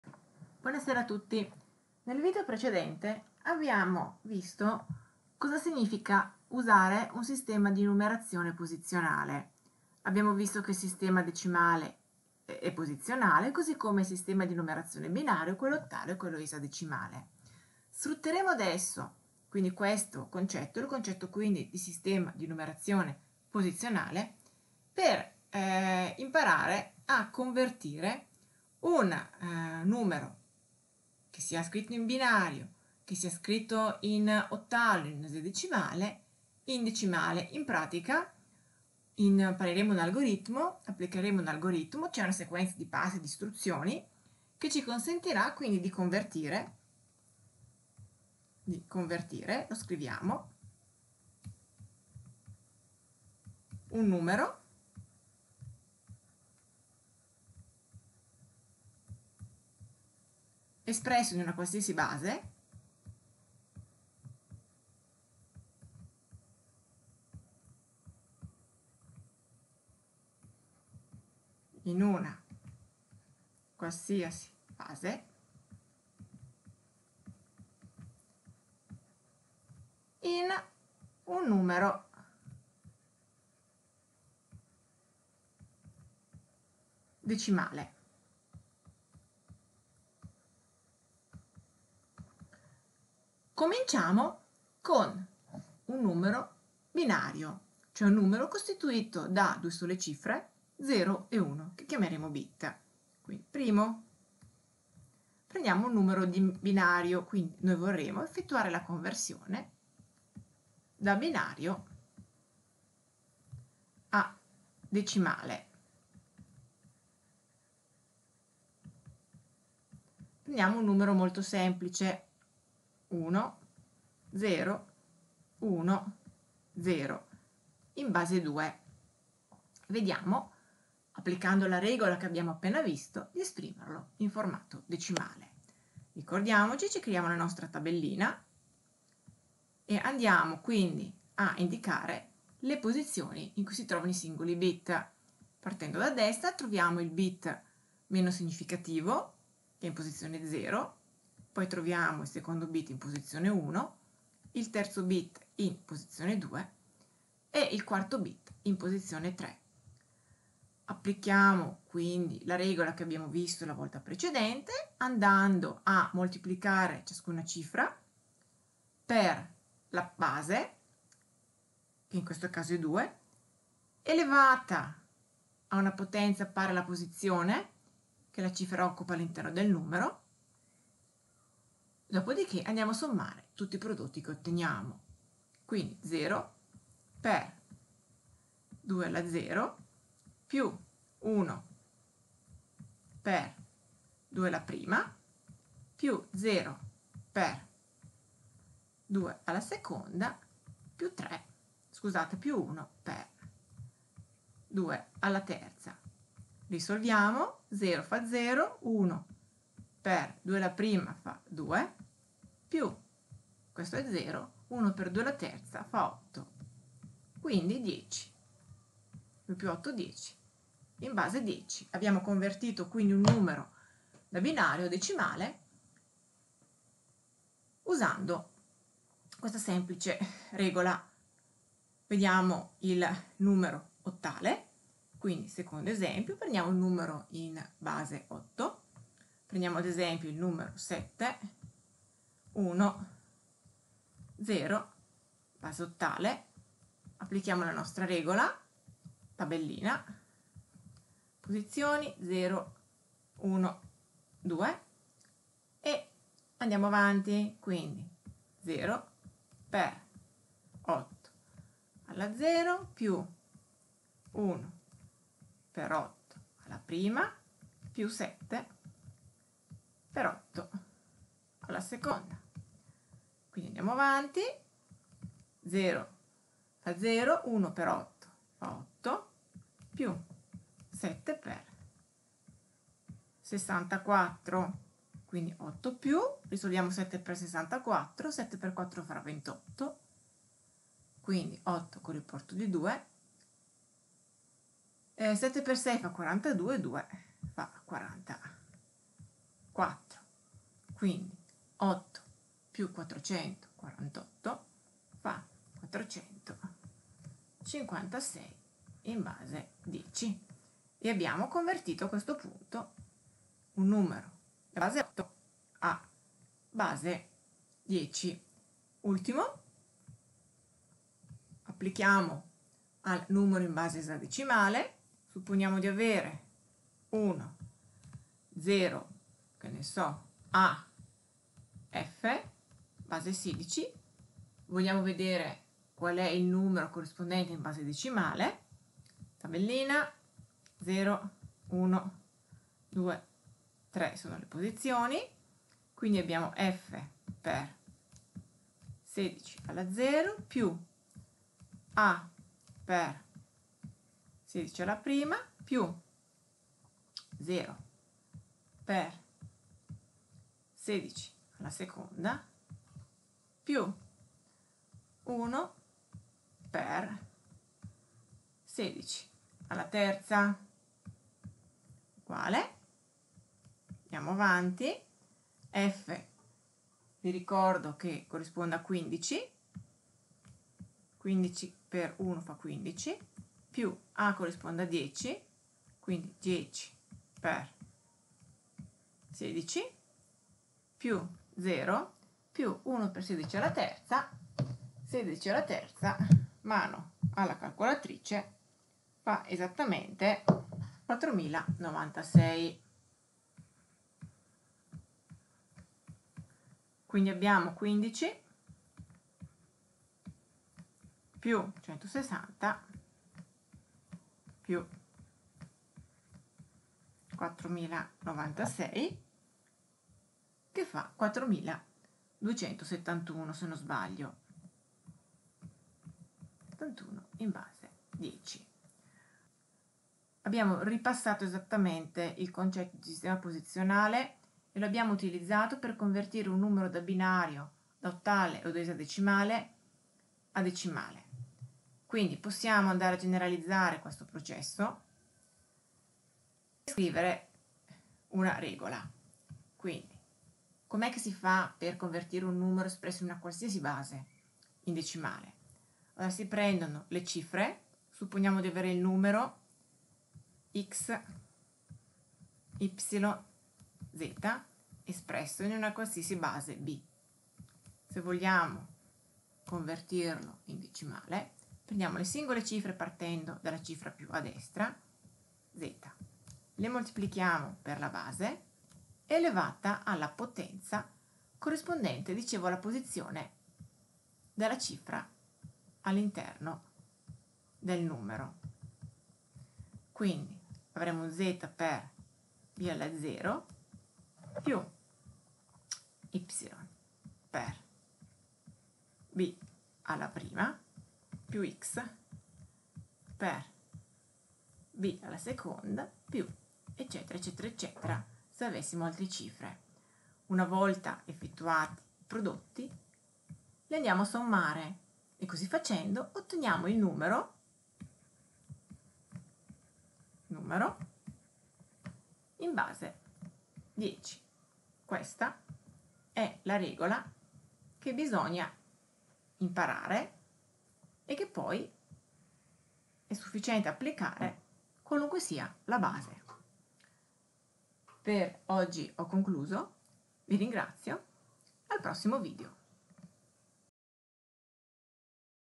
Tecnologie Informatiche. Conversione di un numero binario, ottale, esadecimale in base decimale.
In questo video individueremo l'algoritmo da applicare per convertire un numero espresso in una qualsiasi base in un numero decimale.